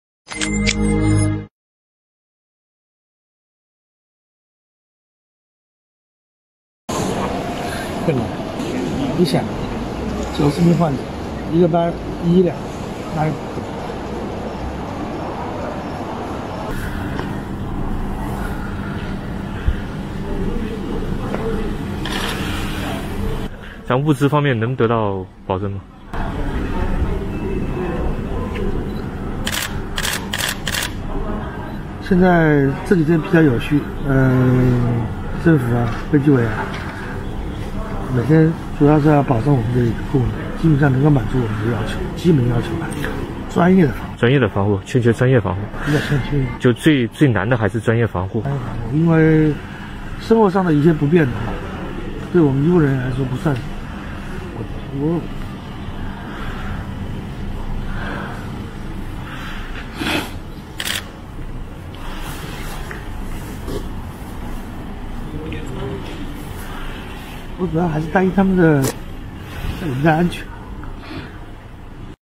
被问有何困难，集中隔离点负责人落泪了
2020年02月04日